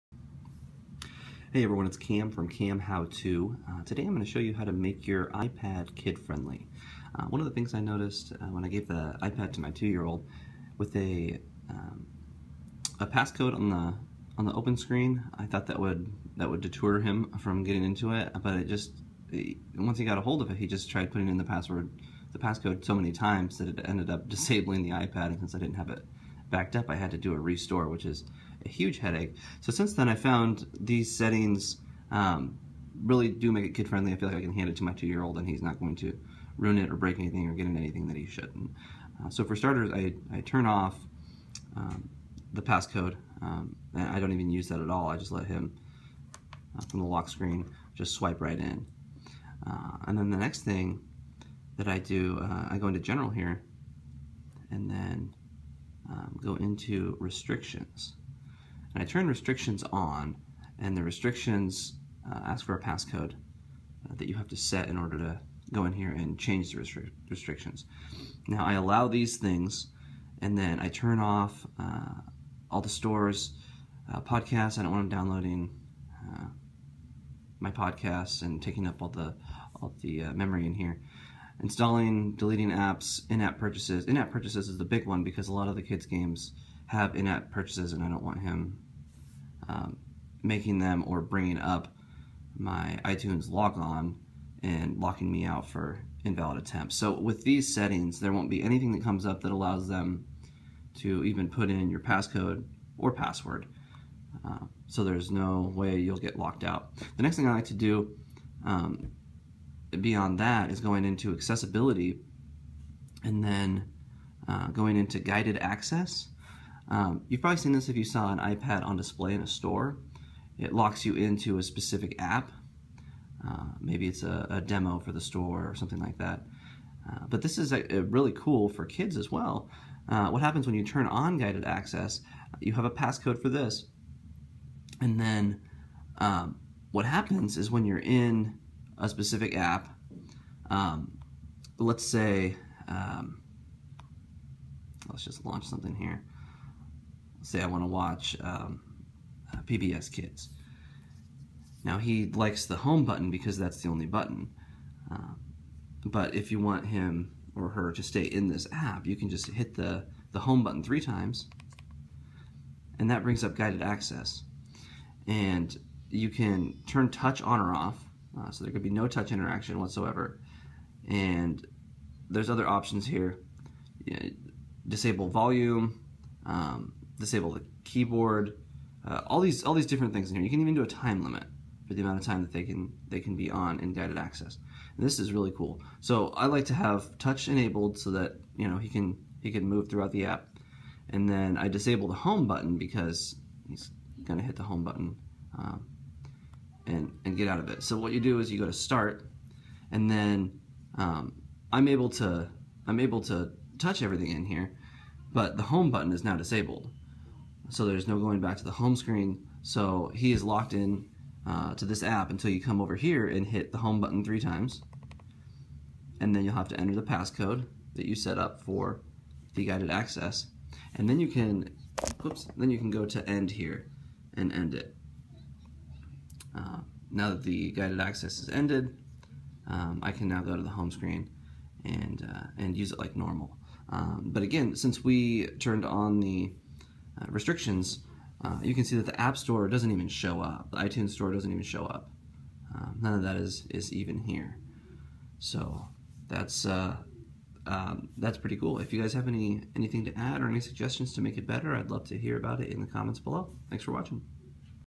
How to Make Your iPad Kid Friendly - CamHow
Out of the box the iPad is not very kid friendly. Kids can buy apps, make in app purchases, and even delete apps and change settings. This video will walk you through necessary steps so you don't have to worry about any of these things when you hand your iPad to a child.